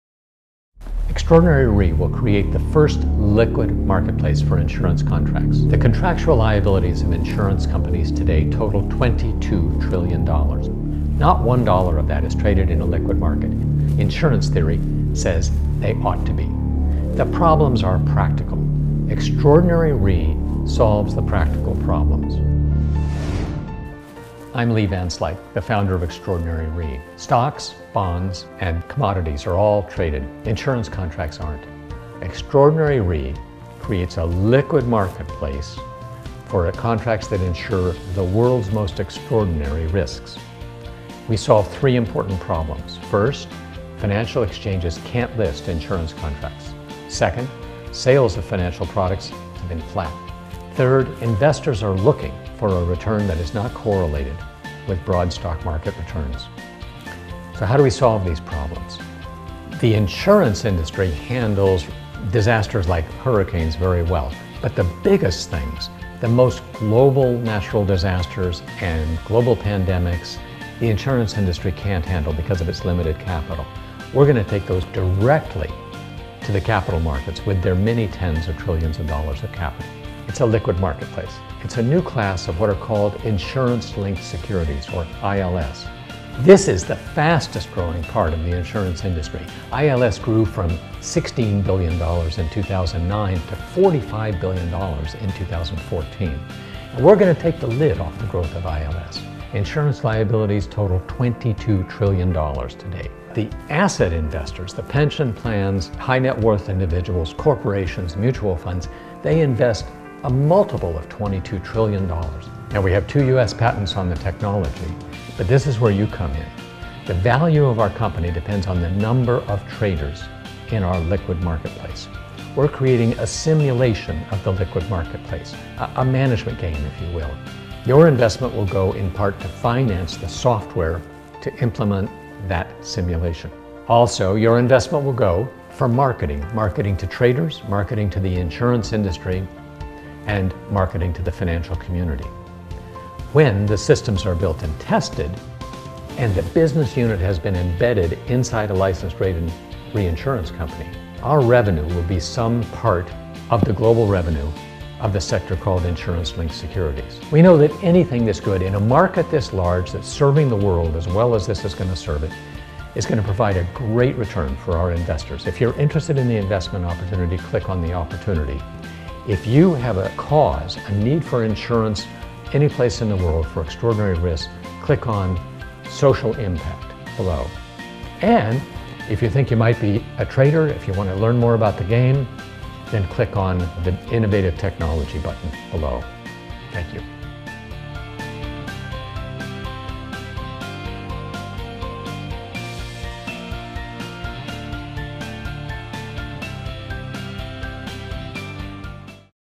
Extraordinary Re - (Reinsurance) The First Liquid Market Place for Insurance Contracts - ILS
Extraordinary Re (reinsurance) is the first liquid market place for contracts that have never been traded before. We solve 3 important problems.

First financial exchanges can’t list insurance contracts. Sales of insurance products have been flat, but there are new demands for insurance products across the globe. Investors are looking for returns that are not correlated with broad stock market returns. So how do you solve these problems?

Extraordinary Re (reinsurance) creates a liquid market place for contracts that insure the world’s most extraordinary risks. On the inside it’s a liquid market place. Shares of contracts are traded. On the outside it’s a new class of what are called Insurance Linked securities or ILS. ILS is the fast growing part of the insurance industry. ILS grew from $16 Billion in 2009 to $45B in 2014. This is our market.

We’re going to take the lid off the growth of ILS. Insurance liabilities total $22 Trillion today. Not $1 of that is traded but the asset investors, the pension plans, the high net worth individuals, the corporate mutual funds. - they invest a multiple of that $22 Trillion.

They’re looking for returns that aren’t correlated with board stock market averages. Now we have 2 patents on the technology but this is where you come in. The value of our company depends on the number of traders in our liquid market place. To prove the concept and enroll traders, we’re creating a simulation of a liquid market place; a management game if you will. Here’s how we’ll spend the, money. 50% for the development of the software and 50% for marketing to the insurance industry and the trading community. We know that anything this good for the world in a market this large is going to provide a great return for the investors.

The insurance (reinsurance) industry wants to know that these extraordinary risks are in demand and that people are going to pay insurance premiums for these. So this is where you come in. If you have a cause, a need for insurance, anyplace in the world for Extraordinary risk, click on social impact above.